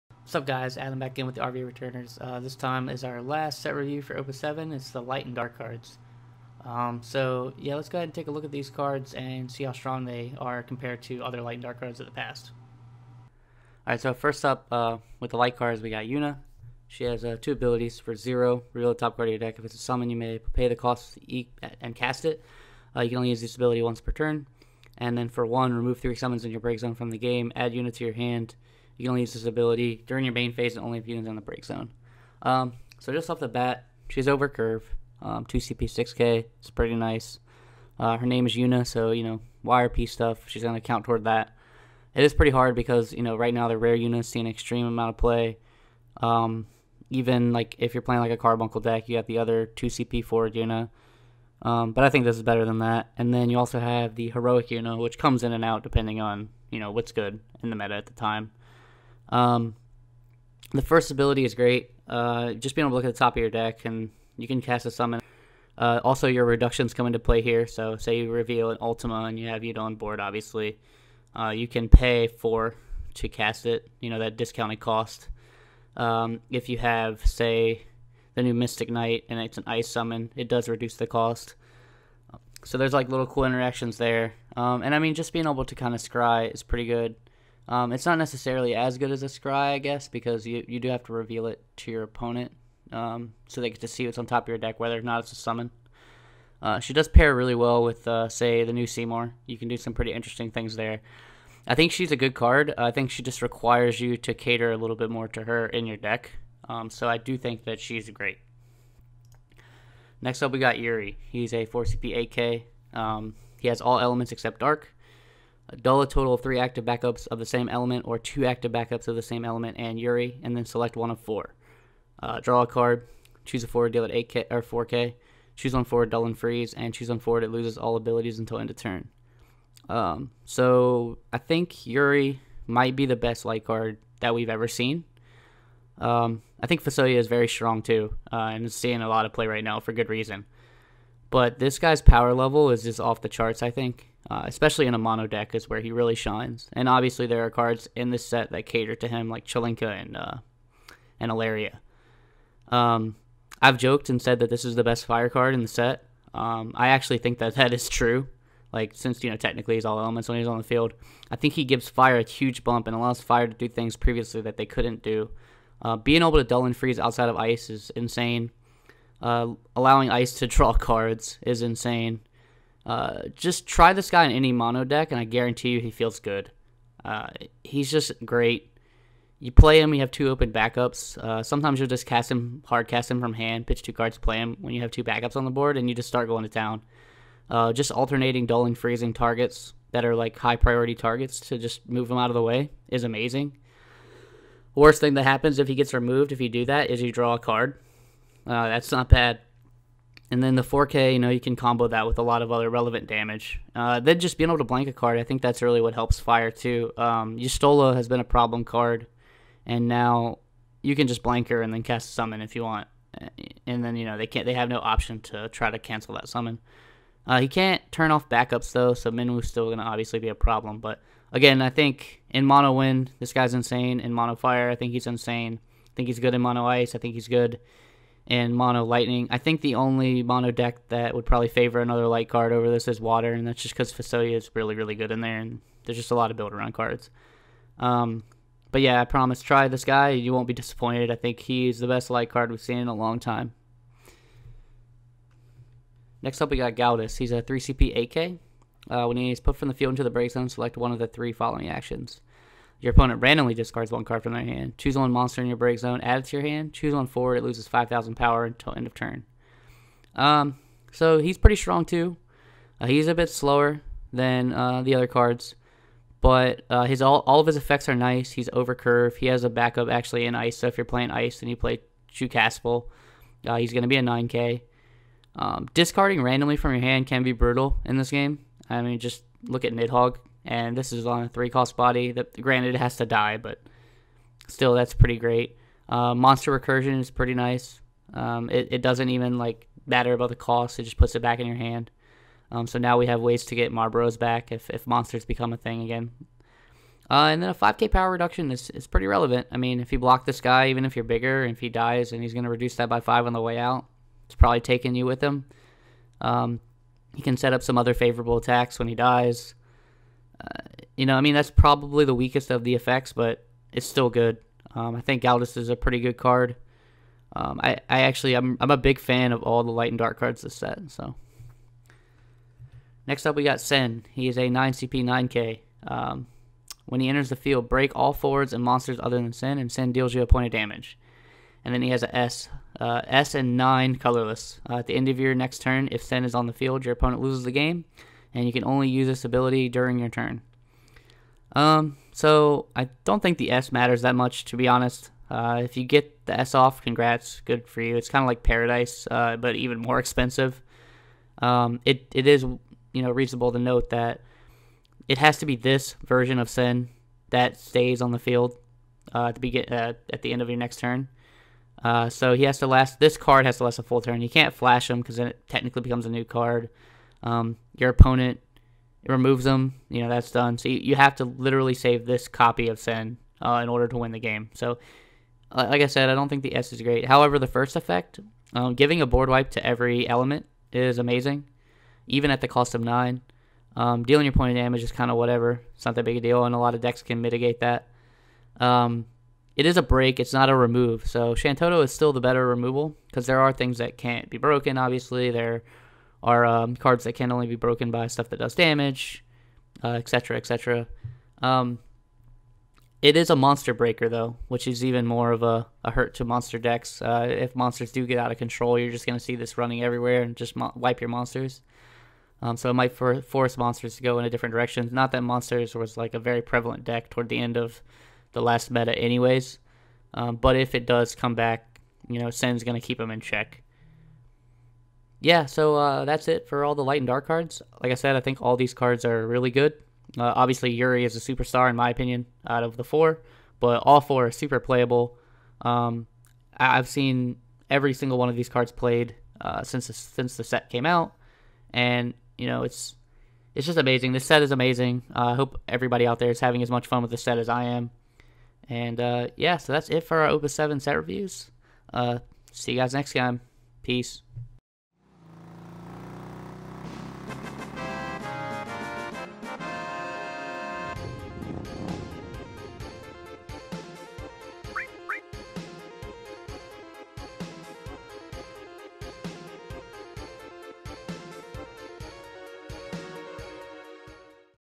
Opus VII Review - Light & Dark | Final Fantasy TCG
RVA Returners Opus VII Light & Dark Review
How is the power level of these light and dark cards?